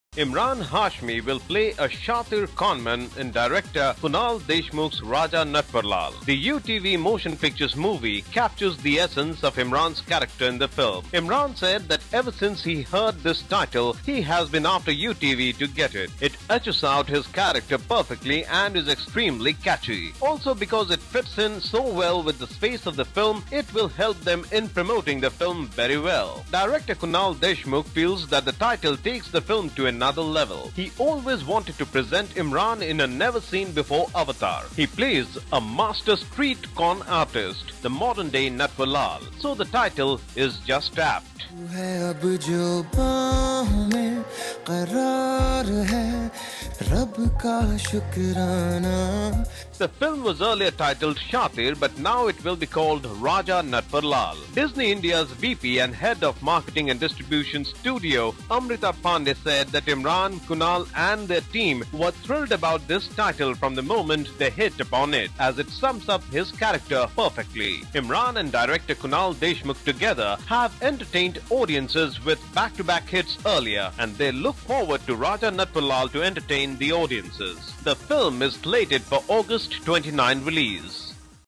Emraan Hashmi is ready to don a new avatar on screen | Bollywood Masala
Watch Latest Bollywood Masala News Emraan Hashmi will play a shaatir conman in director Kunal Deshmukh's Raja Natwarlal. The UTV Motion Pictures' movie captures the essence of Emraan's character in the film.
Emraan said that ever since he heard this title, he has been after UTV to get it. It etches out his character perfectly and is extremely catchy. Also, because it fits in so well with the space of the film, it will help them in promoting the film very well.
Director Kunal Deshmukh feels that the title takes the film to another level. He always wanted to present Emraan in a never seen before avatar. He plays a master street con artist -- the modern day Natwarlal. So the title is just apt.